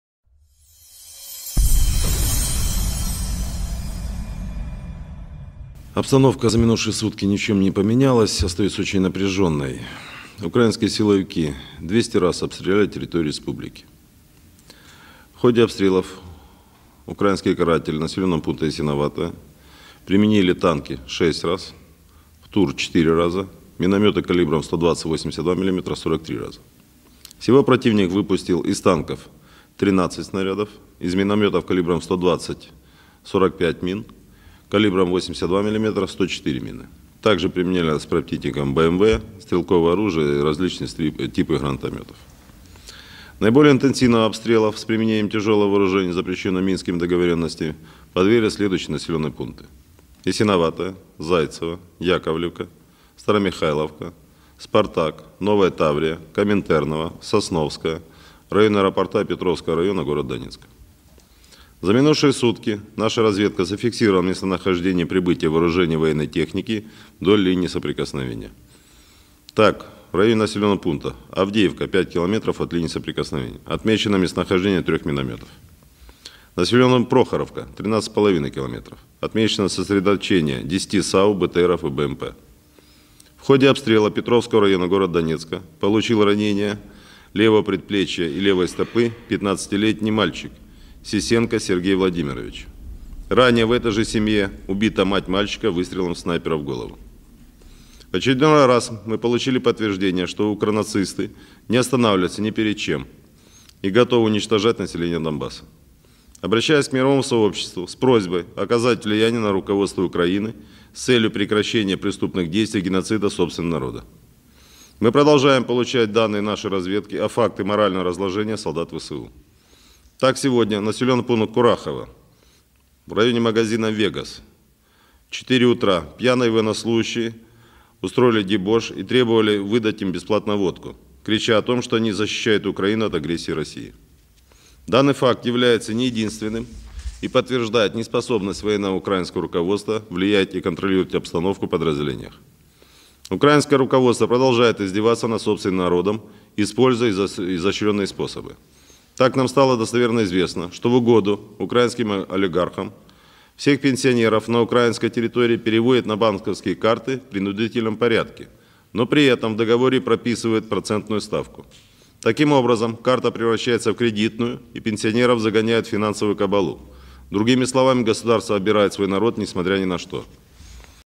Informe del Ministerio de Defensa de la RPD-13 de Marzo 2016 (subtitulos en español)
Informe del Ministerio de Defensa de la RPD-13 de Marzo 2016.
Comandante adjunto del Cuerpo del Ministerio de Defensa de la RPD-Edward Aleksandrovich Basurin.

Kiev trasladó a la línea del frente 10 sistemas autopropulsados de artillería-SAU, morteros y vehículos blindados,-Basurin.

Fecha de publicación: 13 de de marzo 2016, 15:18 (hora local).

La situación durante el último día no ha cambiado nada y sigue siendo muy tensa.

Los agentes ucranianos 200 veces bombardearon el territorio de la República. Durante los bombardeos los castigadores ucranianos en dirección al poblado de Yasinovataya utilizaron tanques (disparos) 6 veces, cohetes guiados antitanques-PTUR-4 veces y morteros de calibre de 120 mm y 82 mm-43 veces.
En total el enemigo disparó desde tanques-13 proyectiles, desde morteros de calibre de 120 mm-45 morteros y de calibre de 82 mm-104 morteros. También el enemigo utilizó vehículos de combate de infantería-BMP, armas de infantería y diferentes tipos de lanzagranadas.

Al más intenso bombardeo con utilización de armamento pesado, prohibido por los acuerdos de Minsk, fueron objeto los siguientes poblados: Yasinovataya, Zaitsevo, Yakovlevka, Staromikhailovka, Spartak, Novaya Tavriya, Kominternovo, Sosnovskoe, el área del aeropuerto y el distrito Petrovsky de la ciudad de Donetsk.

Durante el último día nuestra inteligencia ha registrado la ubicación y la llegada del armamento y equipo militar a la línea de contacto. Así, en las áreas:

Del poblado de Avdeevka (a 5 kilómetros de la línea de contacto) se observó la ubicación de 3 morteros.

En el poblado de Prokhorovka (a 13, 5 kilómetros de la línea de contacto) se observó la concentración de 10 sistemas autopropulsados de artillería-SAU, vehículos blindados-BTR y vehículos de combate de infantería-BMP.

Durante los bombardeos al distrito Petrovsky de la ciudad de Donetsk resulto herido del antebrazo izquierdo y el pie izquierdo un muchacho de 15 años Sergey Vadimovich Sesenko. Anteriormente, en la misma familia murió la madre del muchacho por un disparo de un francotirador en la cabeza. Una vez más nosotros recibimos la confirmación que los ukronazis no se detienen ante nada, y están  dispuestos a destruir a la población del Donbass. Hago un llamamiento a la comunidad internacional, con la solicitud de que tenga impacto en la dirección ucraniana con el objetivo de detener las acciones criminales y el genocidio de su propio pueblo.

Nosotros continuamos recibiendo datos de nuestra inteligencia sobre los hechos de la descomposición moral de los soldados de las fuerzas armadas de Ucrania. Así, hoy en el poblado de Kurakhovo, en la zona de la tienda “Vegas”, a las 4 de la mañana los soldados borrachos exigieron que les regalasen gratis a ellos vodka, gritando que ellos defienden a Ucrania de la agresión de Rusia. El dato de los hechos no es el único y confirma la incapacidad de la dirección militar ucraniana para influir y controlar la situación en las unidades.
La dirección ucraniana continúa riéndose de su propio pueblo utilizando maneras sutiles. Así, a nosotros es auténticamente conocido, que en aras de los oligarcas ucranianos todos los pensionistas en el territorio ucraniano transfieren a las tarjetas bancarias por orden forzada, pero al mismo tiempo en el contrato se establece la tasa de interés. Por lo tanto, la tarjeta se convierte en un crédito y los pensionistas son conducidos a la servidumbre financiera, en otras palabras el estado roba a su pueblo no importando que.